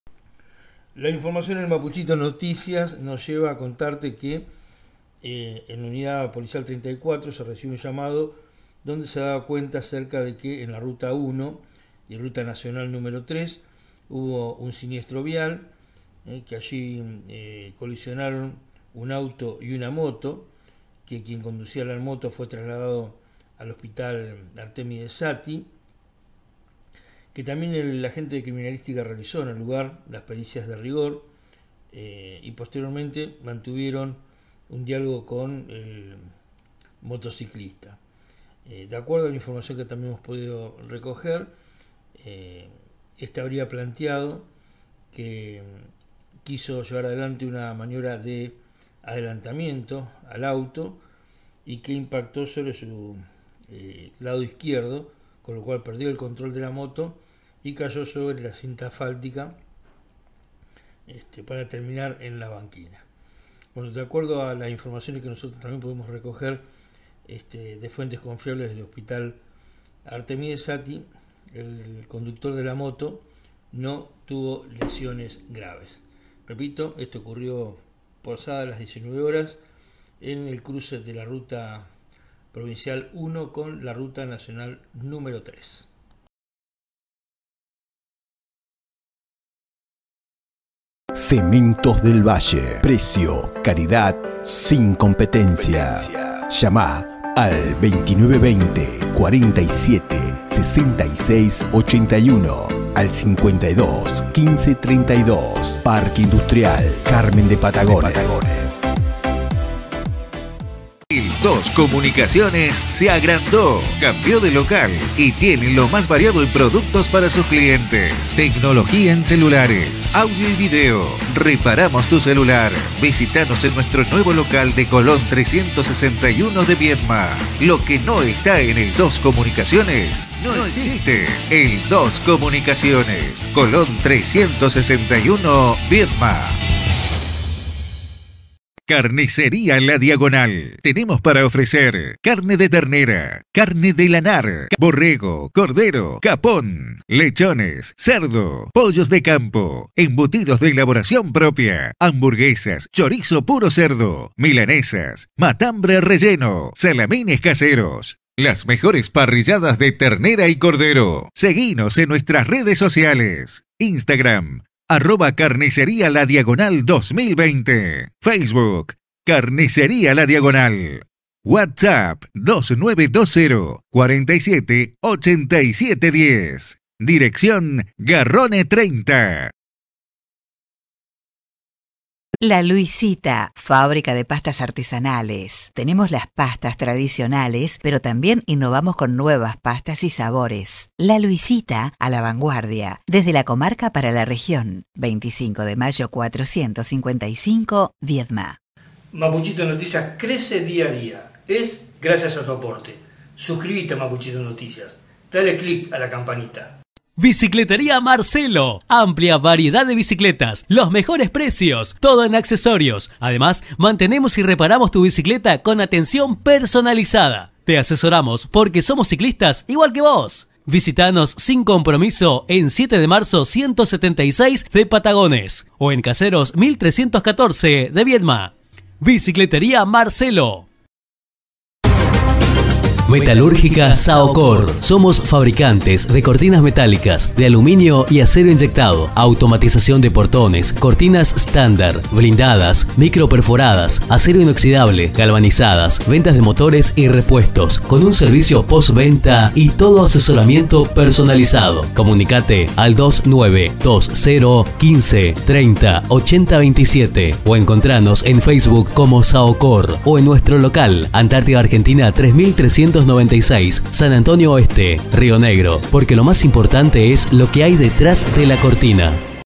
CHOQUE EN RUTA PROVINCIAL 1 Y RUTA NACIONAL 3 CON UN HERIDO LEVE
De acuerdo a la informacion a la que accedio Mapuchito Noticias,  la colision se dio cuando la moto quizo pasar al auto e impacto sobre su lado izquierdo .  FOTO CREDITO NOTICIASNET